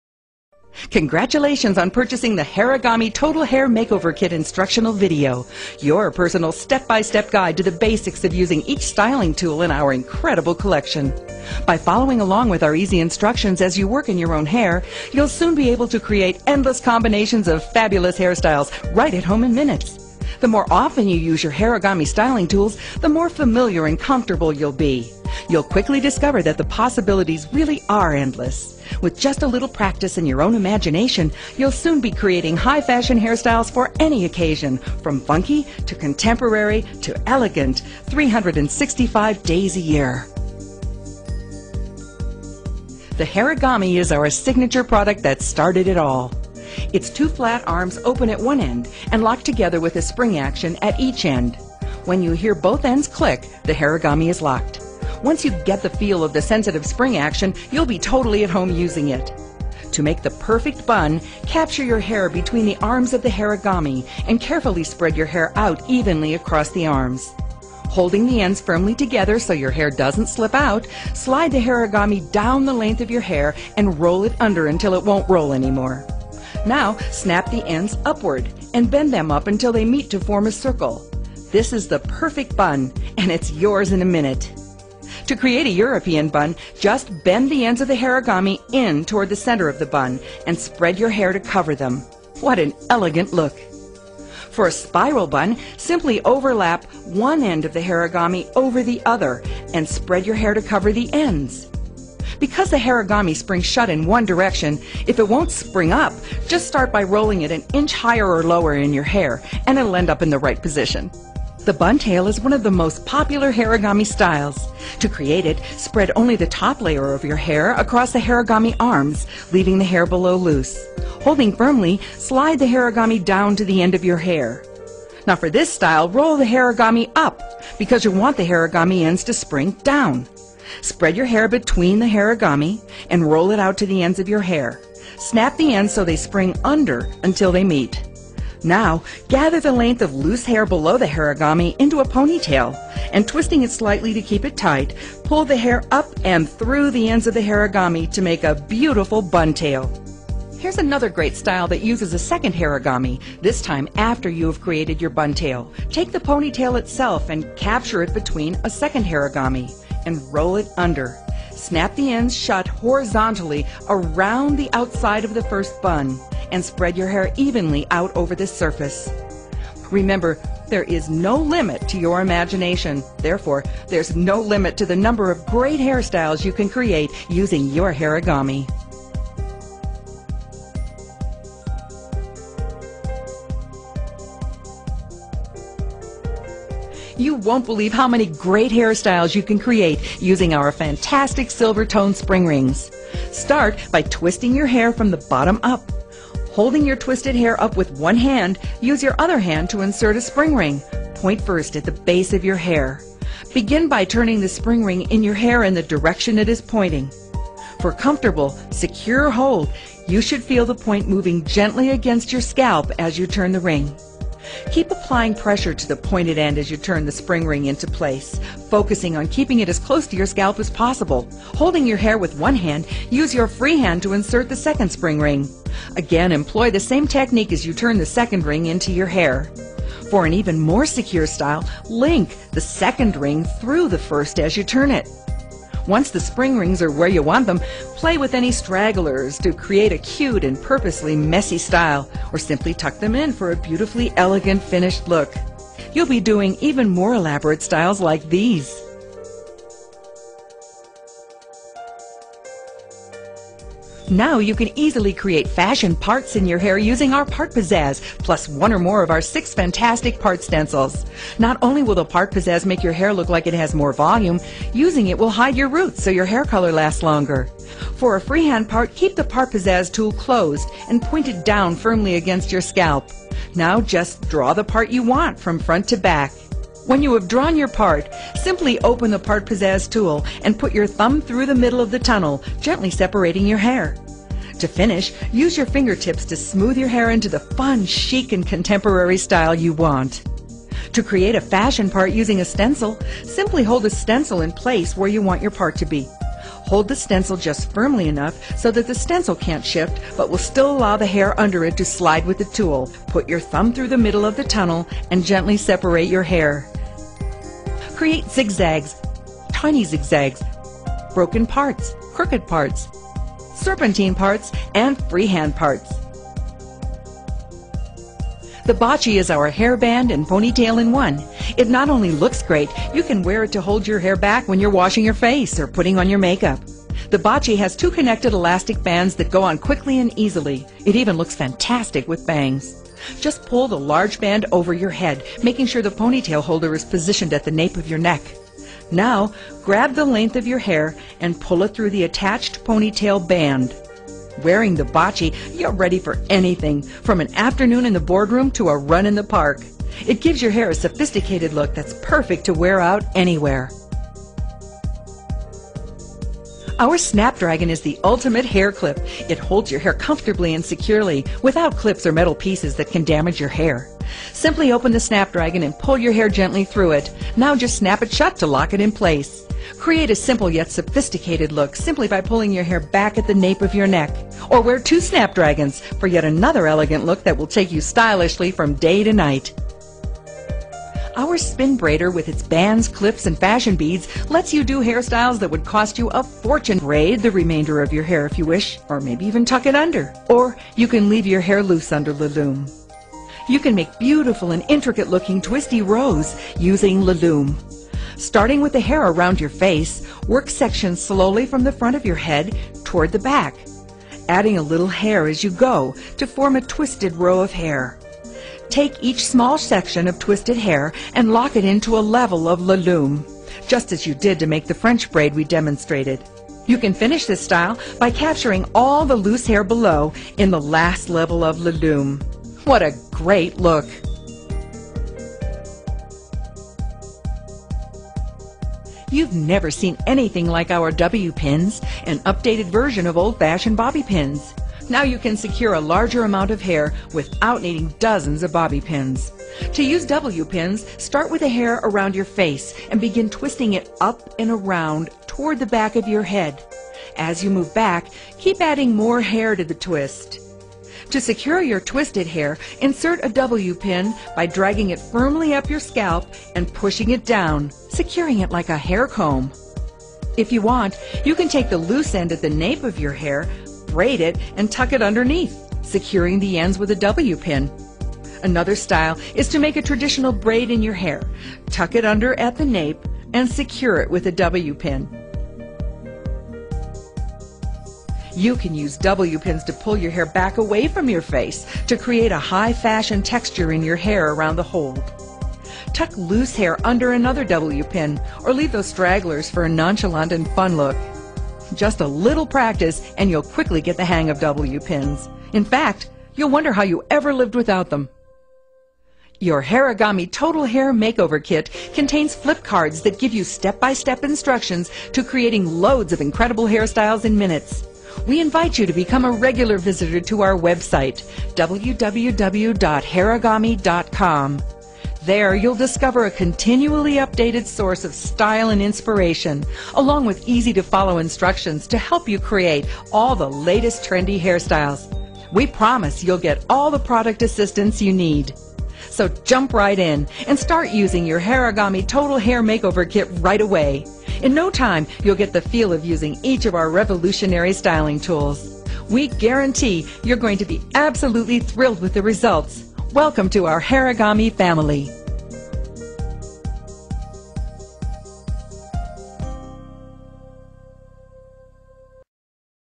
Набор заколок Hairagami
Самые главные плюсы этого набора -- удобство и простота. С заколками Hairagami вы сэкономите уйму времени и сможете выглядеть элегантной и оригинальной. Классический образ деловой леди, романтическая прическа для незабываемого первого свидания, озорные хвостики для девочек-кокеток, самые разные хвосты и собранные пучки, затейливых форм - восьмерки, знака бесконечности, спирали и волны -- на создание всего этого вам потребуется лишь несколько минут. Кроме того, прическа в салоне -- дорогое удовольствие, но с набором заколок для волос Hairagami это перестанет быть для вас проблемой.
Заколки Hairagami — очень оригинальное и выгодное предложение! Супер подарок от настоящего мужчины красивой женщине! В комплект входит DVD-диск с наглядными видео уроками и карточки с иллюстрированными пошаговыми инструкциями по созданию причесок.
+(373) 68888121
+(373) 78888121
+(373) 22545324